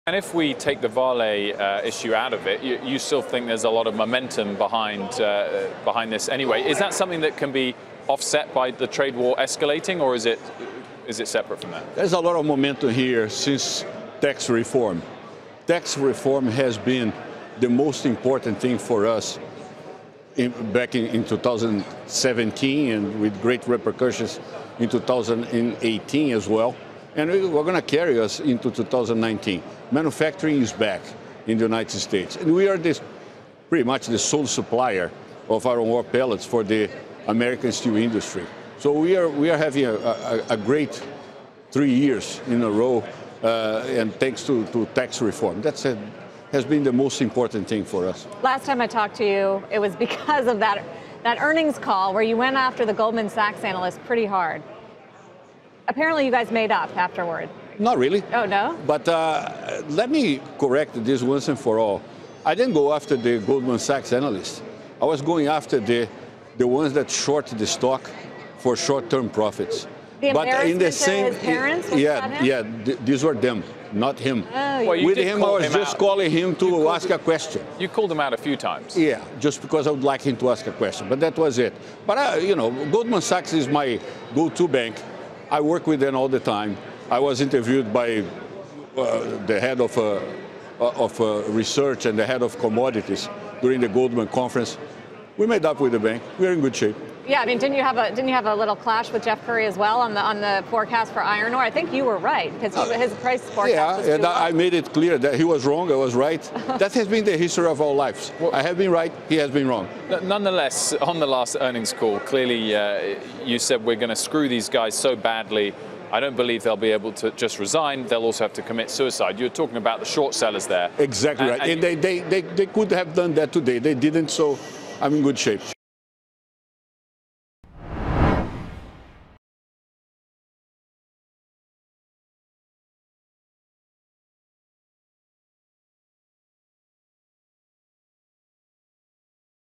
Cleveland-Cliffs CEO: Lots of momentum since tax reform
Lourenco Goncalves, CEO of Cleveland Cliffs, on his strong quarter, spat with Goldman Sachs, and outlook for 2019.